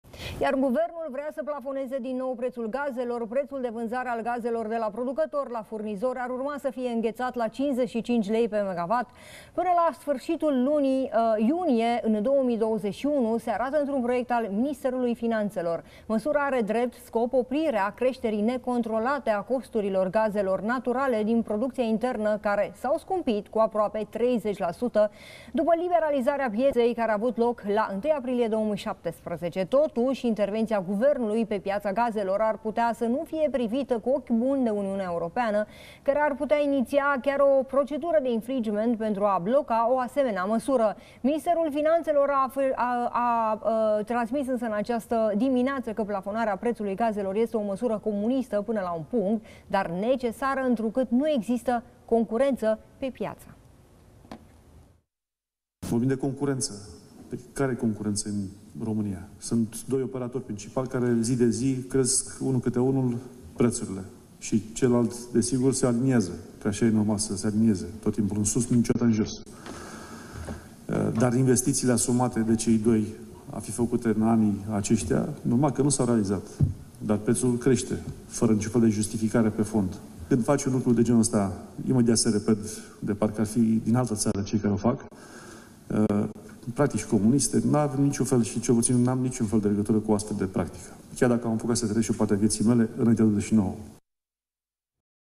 Teodorovici vrea să plafoneze preţul gazelor
Guvernul vrea să plafoneze din nou preţul gazelor. Prețul de vânzare al gazelor de la producători la furnizori ar urma să fie îngheţat la 55 lei/MWh până la sfârşitul lui iunie 2021, se arată într-un proiect al ministerului Finanţelor. Măsura are drept scop oprirea creşterii necontrolate a costurilor gazelor naturale din producţia internă, care s-au scumpit cu aproape 30 la sută după liberalizarea pieţei ce a avut loc la 1 aprilie 2017.